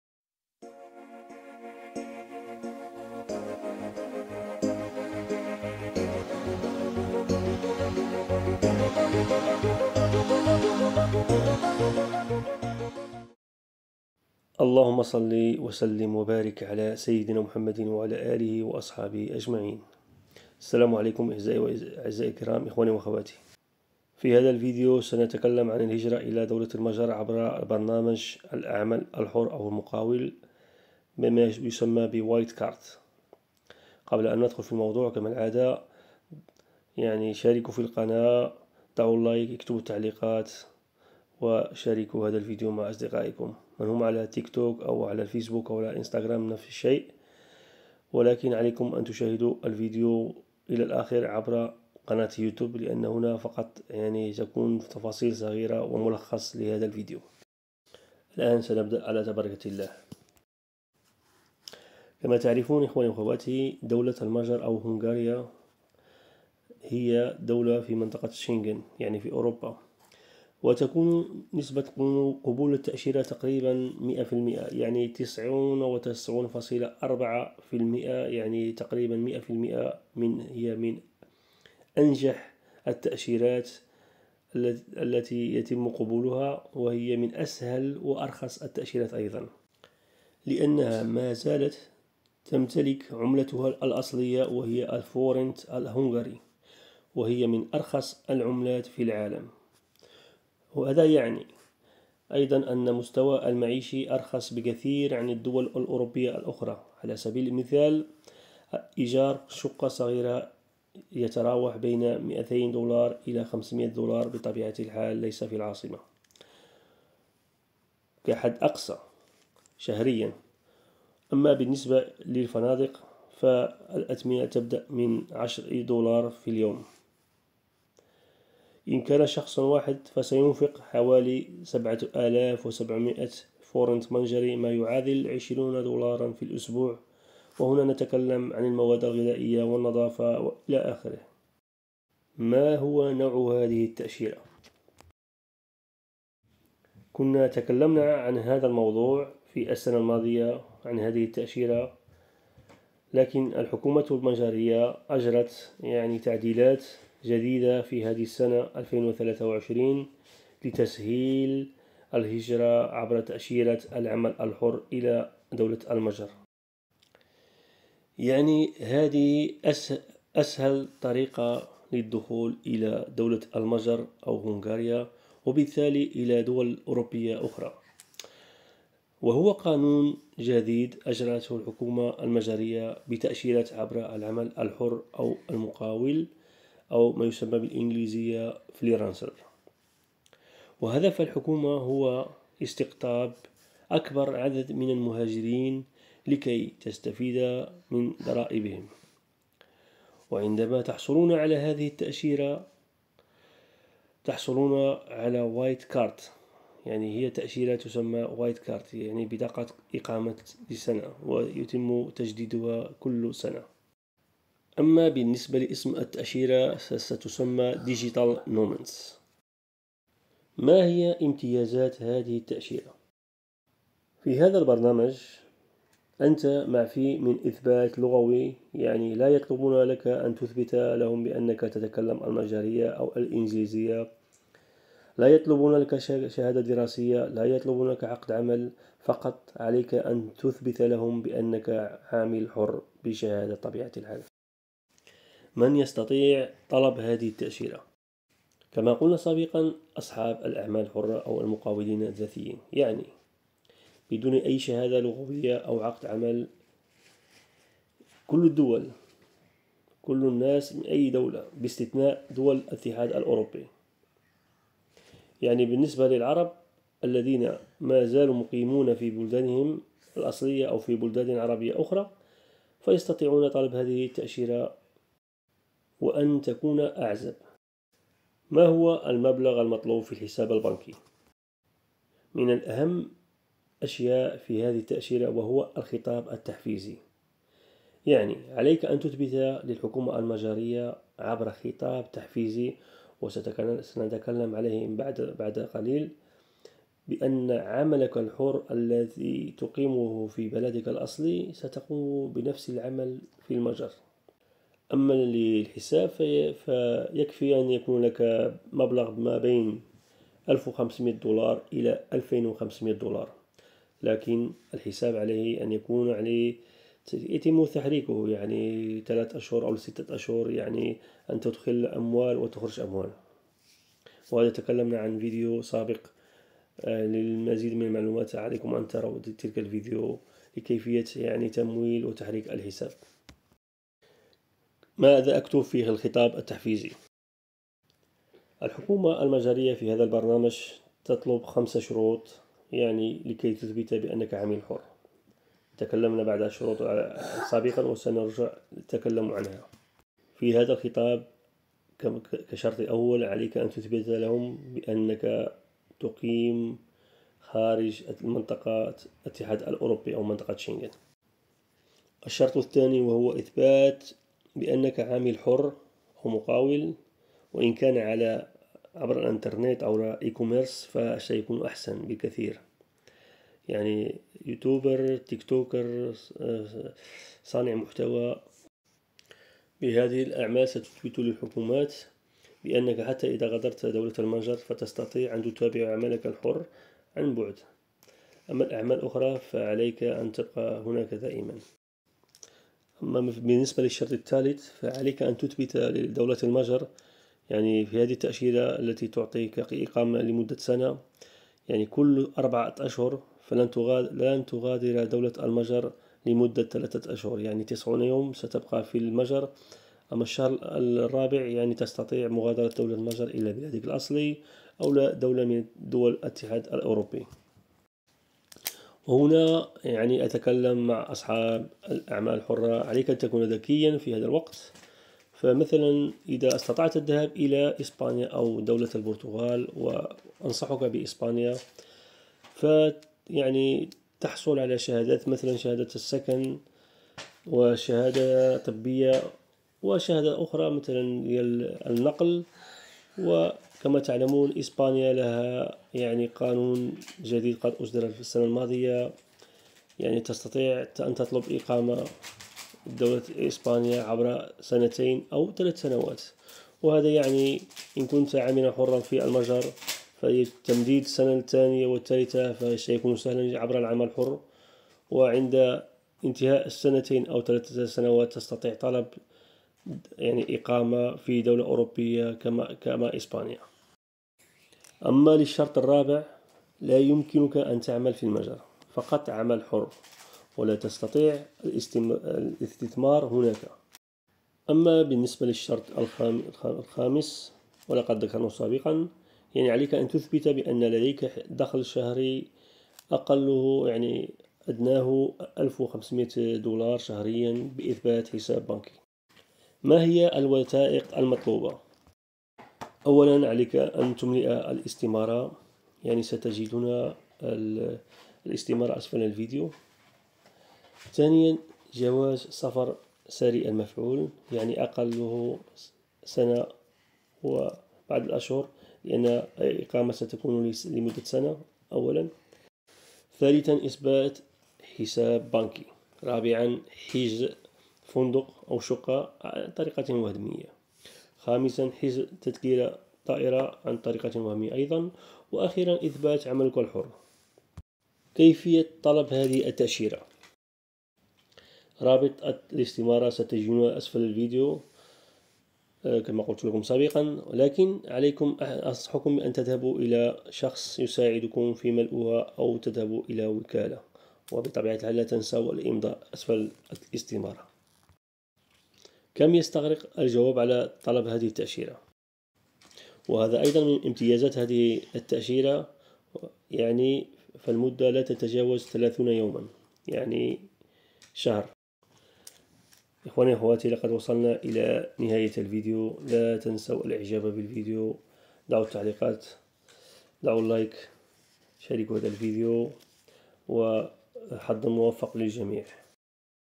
الهجرة_الي_اوروبا عبر تأشيرة دولة المجر كل_العرب يستطيعون الطلب، تأشيرة أصحاب الأعمال الحرة 🇭🇺🇭🇺
السلام عليكم ورحمه الله وبركاته ، اللهم صلي وسلم وبارك على سيدنا محمد وعلى اله وصحبه اجمعين.
إخواني وأخواتي ، في هذا الفيديو سوف نتحدث عن الفيزا بحث دولة المجر، تأشيرة white Card,  تأشيرة لمدة سنتين لي اصحاب المهن الحرة.

سائق شاحنة سائق حافلة سائق سيارة. سائق رافعة شوكية
passive income visa Portugal
visa white Card